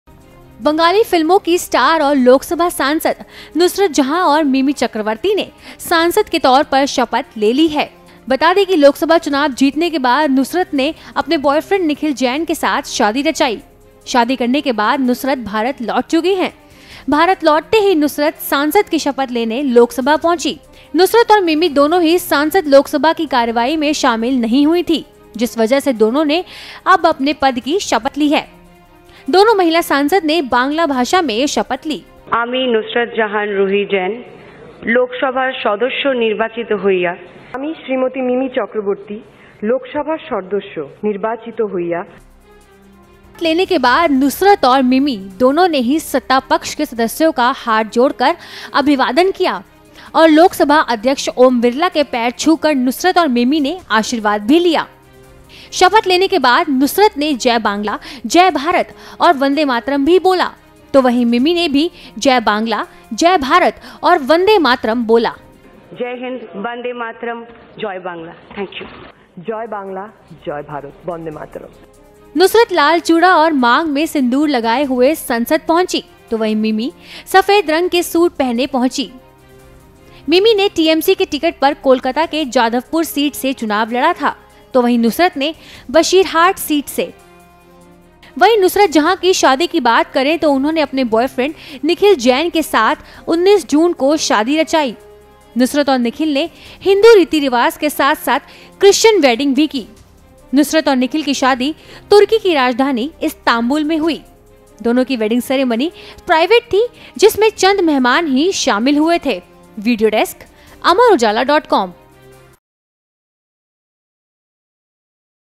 Nusrat Jahan सिंदूर और हाथ में चूड़ा पहने Mimi Chakraborty के साथ पहुंचीं Lok Sabha, सांसद की ली शपथ
Bengali Actress और LokSabha MP Nusrat Jahan और Mimi Chakraborty ने MP के तौर पर oath ले ली है। Oath लेने के बाद दोनों ने  Lok Sabha Speaker Om Birla के पैर छूकर आशीर्वाद लिया। Mimi ने TMC के टिकट पर Jadavpur seat से election लड़ा था तो Nusrat ने Basirhat से।